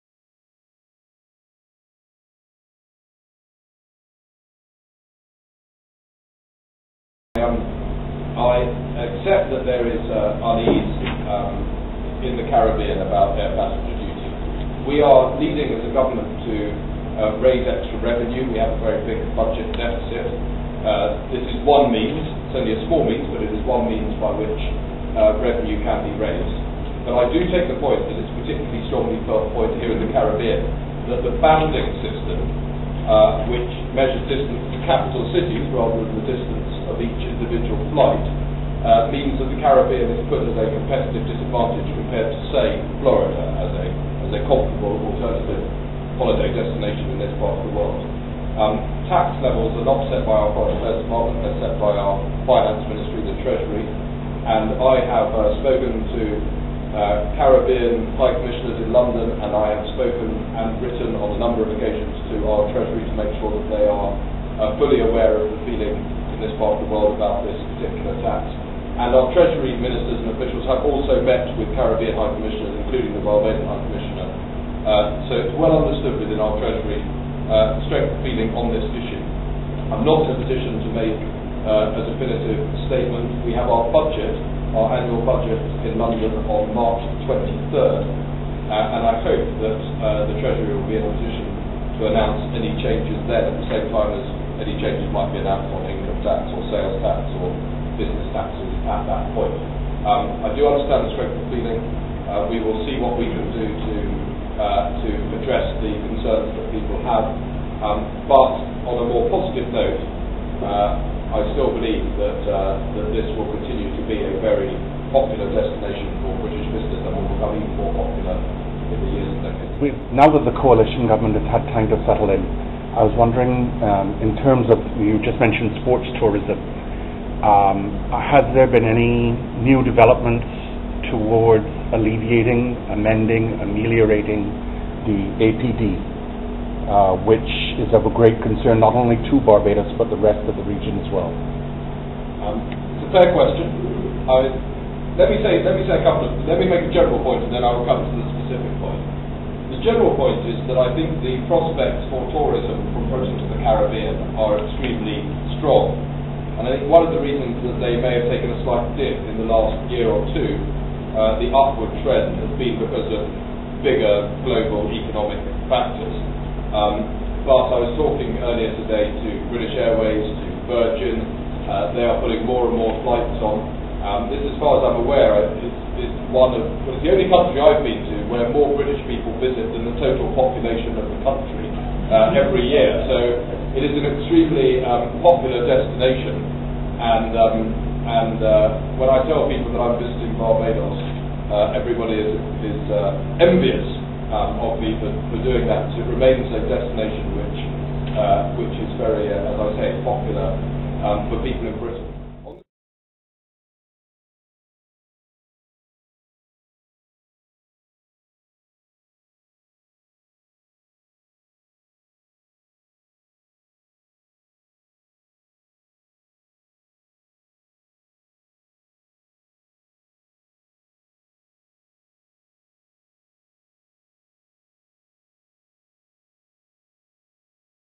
Britain's Foreign Minister in Barbados - Jeremy Browne tackles APD issue
THE battle against the Air Passenger Duty (APD) and its crushing effect on the Caribbean tourism industry continues. Last week, I called for and was granted a special debate in Parliament on the subject. Members of Parliament took part from both the Labour and Conservative parties.
I pointed out that two successive rises in the APD meant that passengers to the Caribbean now pay £75 per person on the duty and passengers in all the other classes paid £150. I explained, as I have set out for Treasury ministers in the past, that the fundamentally iniquitous thing about the APD was the way it was calculated.

I was at pains to point out that everyone supported action to protect the environment and that nobody wanted to deprive the British Treasury of much-needed revenue. But I argued that the method that the UK had chosen to calculate the APD is simply indefensible.